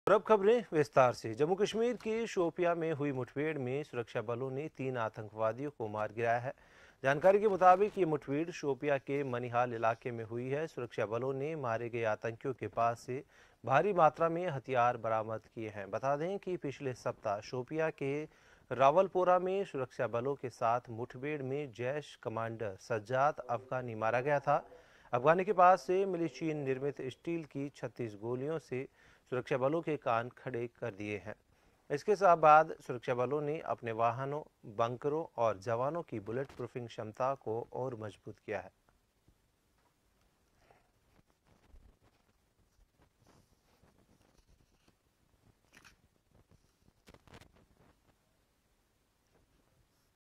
Jammu & Kashmir News | शोपियां में हुई मुठभेड़ में सुरक्षाबलों ने 3 आतंकियों को मार गिराया
News Credit – NRS